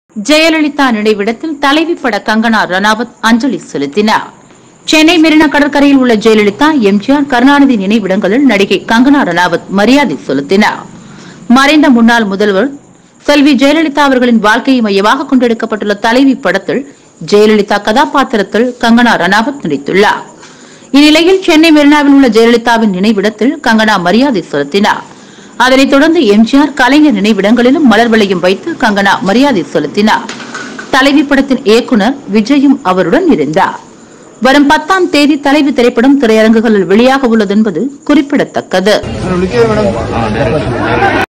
நினைவிடத்தில் 'தலைவி’ கங்கனா ரனாவத் அஞ்சலி...!
சென்னை மெரினா கடற்கரையில் உள்ள ஜெயலலிதா, எம் ஜி ஆர், கருணாநிதி நினைவிடங்களில் நடிகை கங்கனா ரனாவத் மரியாதை செலுத்தினார். மறைந்த முன்னாள் முதல்வர் ஜெயலலிதாஅவின் வாழ்க்கையை மையமாக கொண்டு எடுக்கப்பட்டுள்ள தலைவி படத்தில் ஜெயலலிதா கதாப்பாத்திரத்தில் கங்கனா ரனாவத் நடித்துள்ளார். இந்நிலையில் இன்று சென்னை மெரினாவில் உள்ள ஜெயலலிதா நினைவிடத்தில் கங்கனா மரியாதை செலுத்தினார். அதனை தொடர்ந்து எம் ஜி ஆர், கலைஞர் நினைவிடங்களிலும் மலர் வலையம் வைத்து கங்கனா மரியாதை செலுத்தினார். தலைவி படத்தின் இயக்குனர் விஜயும் அவர் உடன் இருந்தார். வரும் பத்தாம் தேதி தலைவி திரைப்படம் திரையரங்குகளில் வெளியாகவுள்ளது என்பது குறிப்பிடத்தக்கது.